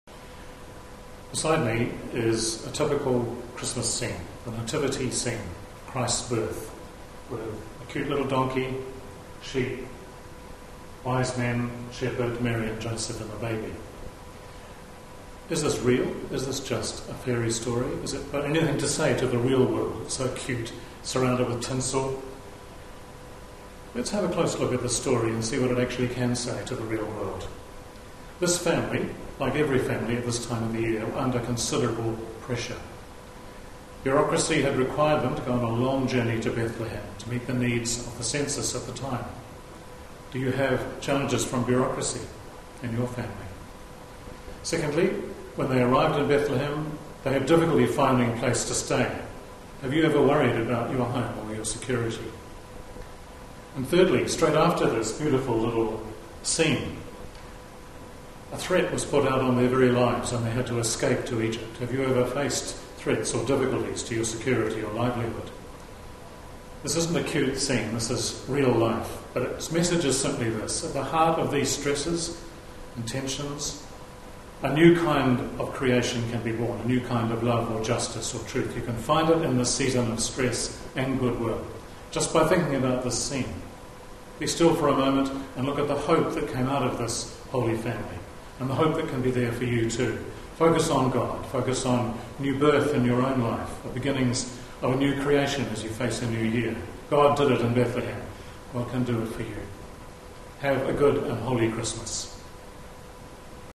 Christmas Message - New Zealand
New Zealand Anglican Archbishop David Moxon shares his Christmas message.